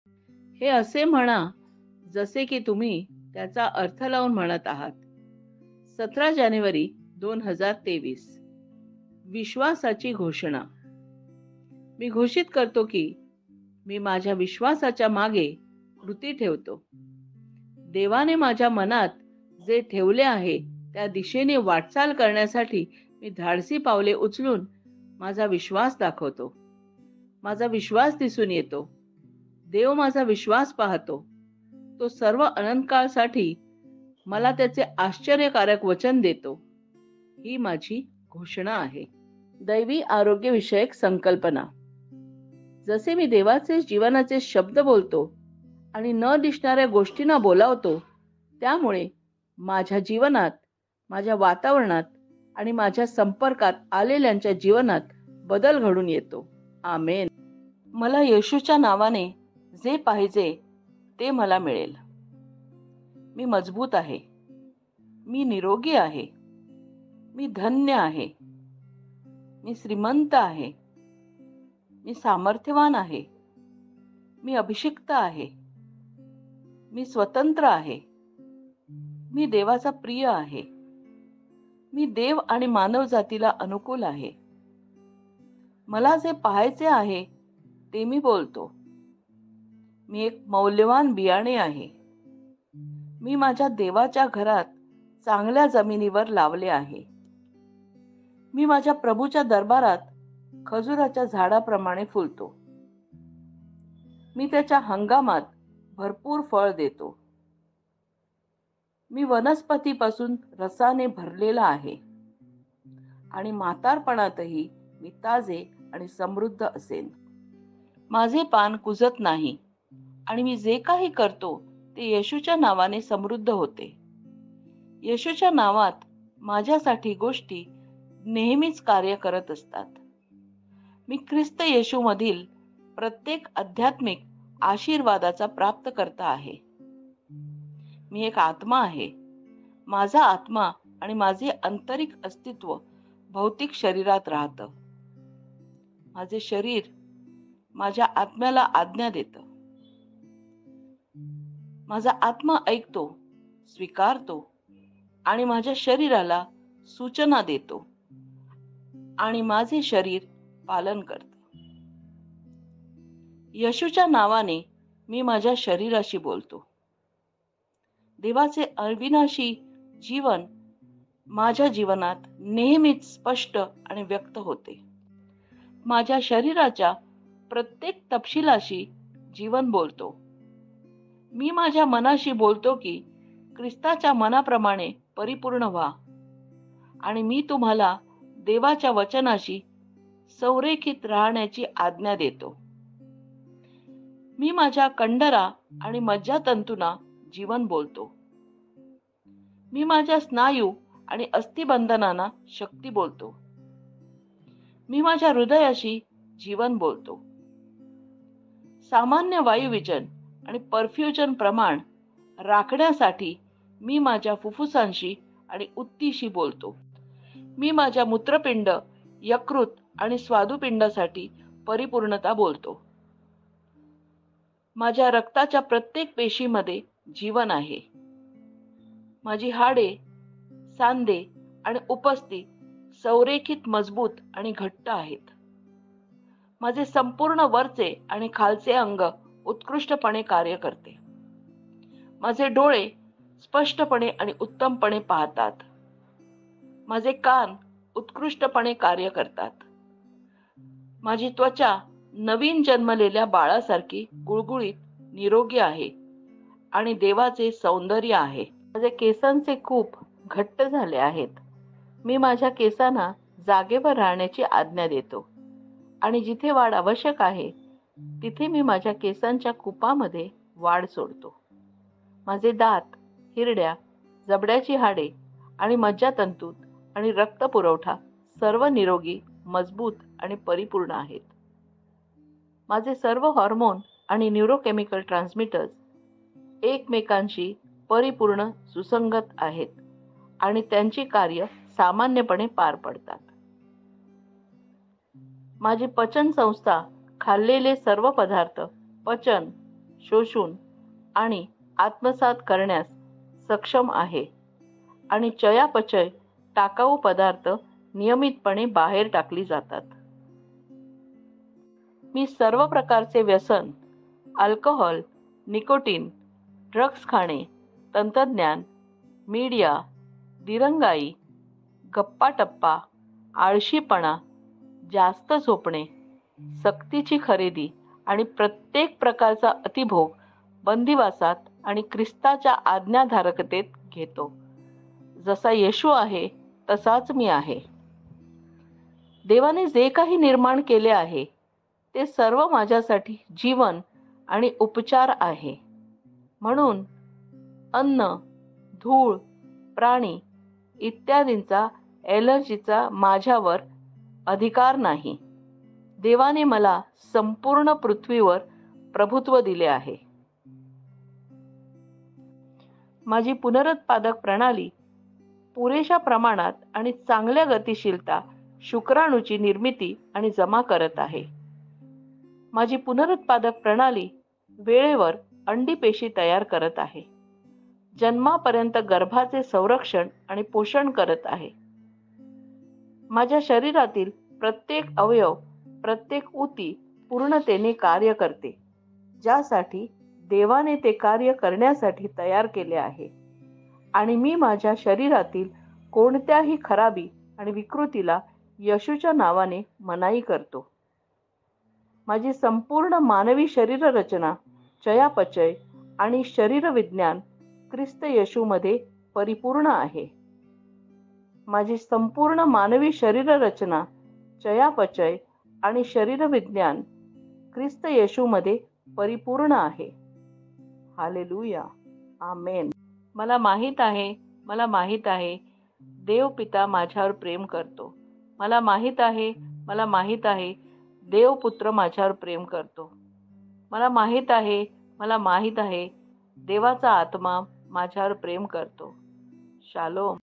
17th January 2023 Say it Like you Mean it (Audio in Marathi)
*हे असे म्हणा जसे की तुम्ही त्याचा अर्थ लावून म्हणत आहात*

 *१७ जानेवारी २०२३*

 *विश्वासाची घोषणा**

मी घोषित करतो की मी माझ्या विश्वासाच्या मागे कृती ठेवतो. *देवाने माझ्या मनात जे ठेवले आहे त्या दिशेने वाटचाल करण्यासाठी मी धाडसी पावले उचलून माझा विश्वास दाखवतो.* माझा विश्वास दिसून येतो. देव माझा विश्वास पाहतो. तो सर्व अनंतकाळासाठी मला त्याचे आश्चर्यकारक वचन देतो. ही माझी घोषणा आहे.

 *दैवी आरोग्यविषयक संकल्पना**
 जसे मी देवाचे जीवनाचे शब्द बोलतो, आणि न दिसणार्‍या गोष्टींना बोलवतो, त्यामुळे माझ्या जीवनात, माझ्या वातावरणात आणि माझ्या संपर्कात आलेल्यांच्या जीवनात बदल घडून येतात *आमेन* !

 *मला येशूच्या नावाने जे पाहिजे ते मला मिळेल  (Jn 14:13)**

मी मजबूत आहे (इफिस ६:१०)
 मी निरोगी  आहे (1 पेट 2:24)
 मी धन्य आहे (इफिस १:३)
 मी श्रीमंत आहे (2 Cor 8:9)
 मी सामर्थ्यवान आहे (40:29 आहे)
 मी अभिषिक्त आहे (1 Jn 2:20)
 मी स्वतंत्र आहे (Jn ८:३६)
 मी देवाचा प्रिय आहे (Mk 1:11)
 मी देव आणि मानवजातीला अनुकूल आहे  (उत्पत्ति 39:2-3)
 मला जे पहायचे आहे ते मी बोलतो (2 Cor 4:18)
 मी एक मौल्यवान बियाणे आहे (लूक ८:१५)
 मी माझ्या देवाच्या घरात चांगल्या जमिनीवर लावले आहे (Ps 92:13)
 मी माझ्या प्रभूच्या दरबारात खजुराच्या झाडाप्रमाणे फुलतो (Ps 92:13)
 मी त्याच्या हंगामात भरपूर फळ देतो (Ps 1:3)
  मी वनस्पती पासून रसाने भरलेला आहे (Ps 92:14)
 आणि म्हातारपणातही मी ताजे आणि समृद्ध असेन (Ps 92:14)
 माझे पान कुजत नाही आणि मी जे काही करतो ते येशूच्या नावाने समृद्ध होते (Ps 1:3)
 येशूच्या नावात माझ्यासाठी गोष्टी नेहमीच कार्य करत असतात (रोम ८:२८)
 मी ख्रिस्त येशूमधील प्रत्येक आध्यात्मिक आशीर्वादाचा प्राप्तकर्ता आहे (इफिस 1:3)

 *मी* *एक* *आत्मा* *आहे*
 *माझा* *आत्मा*  *आणि* *माझे आंतरिक* *अस्तित्वात* *भौतिक शरीरात राहतं*
 **माझं शरीर माझ्या आत्म्याला आज्ञा देतं.  माझा आत्मा ऐकतो, स्वीकारतो आणि माझ्या शरीराला सूचना देतो आणि माझे शरीर पालन करतं.*

 *येशूच्या नावाने, मी माझ्या शरीराशी बोलतो*
 *देवाचे अविनाशी जीवन* माझ्या जीवनात नेहमीच स्पष्ट आणि व्यक्त होते
 *माझ्या शरीराच्या प्रत्येक तपशिलासाठी जीवन बोलतो* .
✝️मी माझ्या मनाशी बोलतो की ख्रिस्ताच्या मनाप्रमाणे *परिपूर्ण* *व्हा* आणि मी तुम्हाला देवाच्या वचनाशी *संरेखित राहण्याची* आज्ञा देतो
✝️ मी माझ्या कंडरा आणि मज्जातंतूंना *जीवन* बोलतो
 ✝️ मी माझ्या स्नायू आणि अस्थिबंधनांना *शक्ती* बोलतो
 ✝️ मी माझ्या हृदयाशी *जीवन* बोलतो
 ✝️ **सामान्य वायुवीजन आणि परफ्यूजन प्रमाण * राखण्यासाठी* मी माझ्या फुफ्फुसांशी आणि ऊतींशी बोलतो
 ✝️ मी माझ्या मूत्रपिंड, यकृत आणि स्वादुपिंडासाठी *परिपूर्णता* बोलतो
 ✝️ माझ्या **रक्ताच्या प्रत्येक पेशीमध्ये जीवन आहे*
 ✝️ माझी हाडे, सांधे आणि उपास्थि *संरेखित, मजबूत आणि घट्ट* आहेत
 ✝️ माझे संपूर्ण वरचे आणि खालचे अंग *उत्कृष्टपणे कार्य करते*
 ✝️ माझे डोळे *स्पष्टपणे आणि उत्तमपणे पाहतात**
 ✝️ माझे कान *उत्कृष्टपणे कार्य करतात*
 ✝️ माझी त्वचा नवीन जन्मलेल्या बाळासारखी **गुळगुळीत, निरोगी** आहे आणि देवाचे सौंदर्य आहे
 ✝️ माझ्या केसांचे कूप *घट्ट* झाले आहेत.* मी माझ्या केसांना **जागेवर राहण्याची* आज्ञा देतो आणि जिथे वाढ आवश्यक आहे तिथे मी माझ्या *केसांच्या कूपांमध्ये वाढ सोडतो .*
 ✝️ माझे दात, हिरड्या, जबड्याची हाडे आणि मज्जातंतू आणि रक्तपुरवठा सर्व *निरोगी, मजबूत आणि परिपूर्ण* आहेत
 ✝️ माझे सर्व हॉर्मोन आणि न्यूरोकेमिकल ट्रान्समीटर *एकमेकांशी परिपूर्ण सुसंगत* आहेत आणि त्यांची कार्ये सामान्यपणे पार पाडतात
 ✝️ माझी पचनसंस्था खाल्लेले सर्व पदार्थ *पचन, शोषून आणि आत्मसात करण्यास सक्षम आहे* आणि चयापचय *टाकाऊ पदार्थ  नियमितपणे बाहेर टाकली जातात**
 ✝️ मी सर्व प्रकारचे *व्यसन* (अल्कोहोल, निकोटीन, ड्रग्ज) खाणे, तंत्रज्ञान, मेडिया दिरंगाई गप्पाटप्पा, आळशीपणा, जास्त झोपणे, सक्तीची खरेदी आणि प्रत्येक प्रकारच्या *अतिभोग* .  *बंदिवासात आणि ख्रिस्ताच्या आज्ञाधारकतेत* *घेतो* .  *जसा येशू आहे तसा मी आहे*

 ✝️ देवाने जे काही निर्माण केले आहे ते सर्व माझ्यासाठी जीवन आणि उपचार आहे, म्हणून *अन्न, धूळ, प्राणी इत्यादींच्या ऍलर्जीचा माझ्यावर अधिकार नाही.* देवाने मला संपूर्ण पृथ्वीवर प्रभुत्व दिले आहे
 ✝️ 🙎🏻‍♂️ ♂️ माझी पुनरुत्पादक प्रणाली पुरेशा प्रमाणात आणि चांगल्या गतिशीलता शुक्राणूंची निर्मिती आणि जमा करत आहे
✝️🙍🏻‍♀️ ‍♀️ माझी पुनरुत्पादक प्रणाली वेळेवर अंडी पेशी तयार करत आहे, जन्मापर्यंत गर्भाचे संरक्षण आणि पोषण करत आहे
✝️ माझ्या शरीरातील प्रत्येक अवयव, प्रत्येक ऊती पूर्णतेने कार्य करते ज्यासाठी देवाने ते कार्य करण्यासाठी तयार केले आहे, आणि *मी माझ्या शरीरातील कोणत्याही खराबी आणि विकृतीला येशूच्या नावाने मनाई करतो.*
✝️ माझी संपूर्ण मानवी शरीररचना, चयापचय आणि शरीरविज्ञान ख्रिस्त येशूमध्ये परिपूर्ण आहे

 हल्लेलुया! आमेन.

 *मला माहीत आहे मला माहीत आहे* *देव पिता, माझ्यावर प्रेम करतो* ,
 * *मला माहीत आहे मला माहीत आहे* *देव  पुत्र, माझ्यावर प्रेम करतो.*
 * *मला माहीत आहे मला माहीत आहे* *देवाचा आत्मा माझ्यावर प्रेम करतो,**

 शालोम